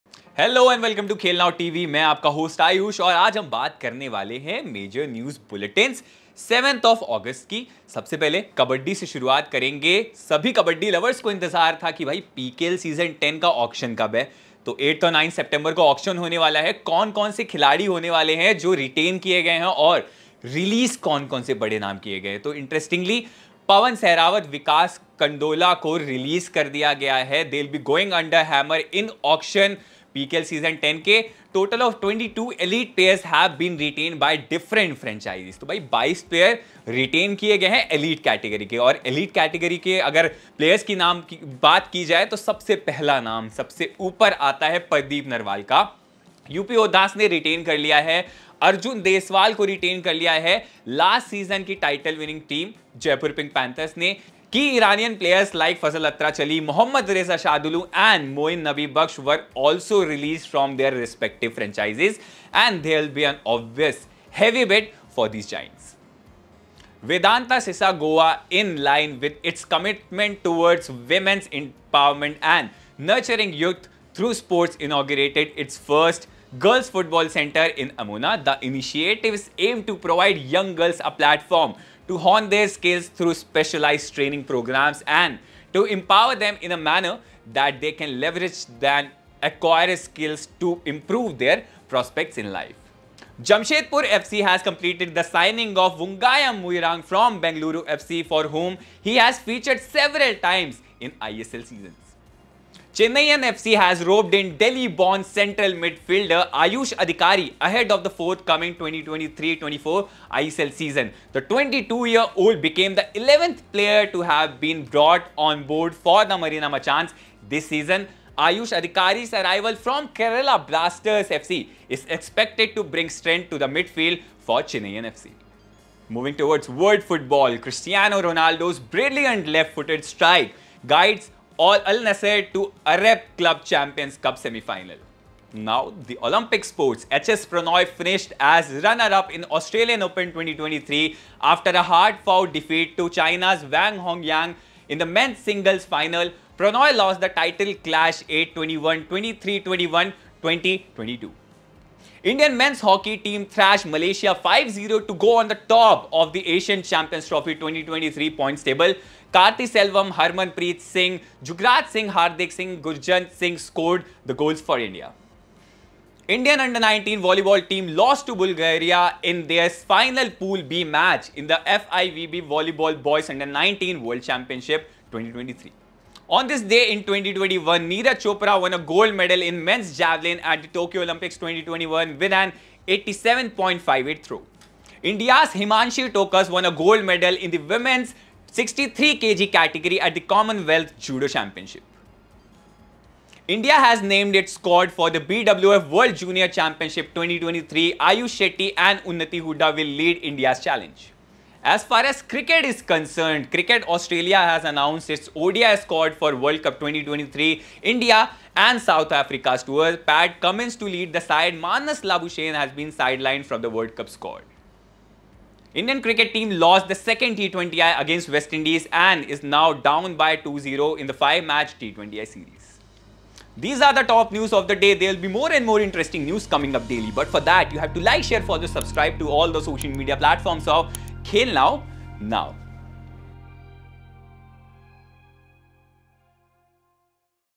Pardeep Narwal Retained, Maninder & Pawan Released | India 5-0 Malaysia in Hockey | Sports News
UP Yoddhas retained Pardeep Narwal for PKL 10. Pawan Sehrawat, Maninder Singh, Vikas Kandola released. Vedanta Sesa Goa inaugurates their first Girls Football Centre in Amona. Jamshedpur FC has completed the signing of Wungngayam Muirang from Bengaluru FC. Chennaiyin FC has roped in Delhi-born central midfielder Ayush Adhikari ahead of the forthcoming 2023-24 season. Cristiano Ronaldo's Brilliant Left-Footed Strike Guides Al Nassr to Arab Club Champions Cup Semi-Final. Indian men's Hockey team thrashes Malaysia 5-0 to go on top of the Aisan Champion Trophy 2023 points table. Indian U-19 Volleyball team lost to Bulgaria in their final Pool B match in the FIVB Volleyball Boys' U-19 World Championship 2023. On this day in 2021, Neeraj Chopra won a gold medal in men's javelin at the Tokyo Olympics 2021 with an 87.58 throw. Cricket Australia announces the squad for ODI World Cup 2023.